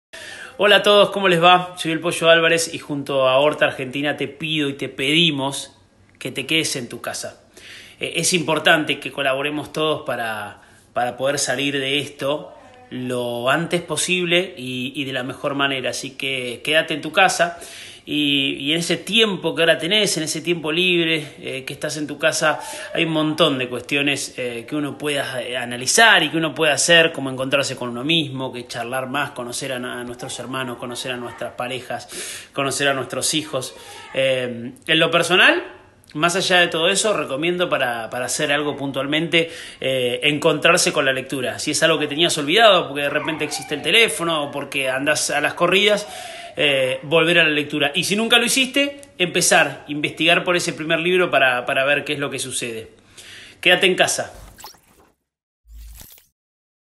ORTenCasa | ¡Ahora es el turno del Pollo Álvarez!
El actor y conductor argentino se suma a la campaña para recordarnos que, si colaboramos todos en conjunto, será más fácil superar esta situación y salir adelante ❤️.

Por eso te pedimos que ¡te quedes en casa! 😉
¡Gracias, Pollo, por el video!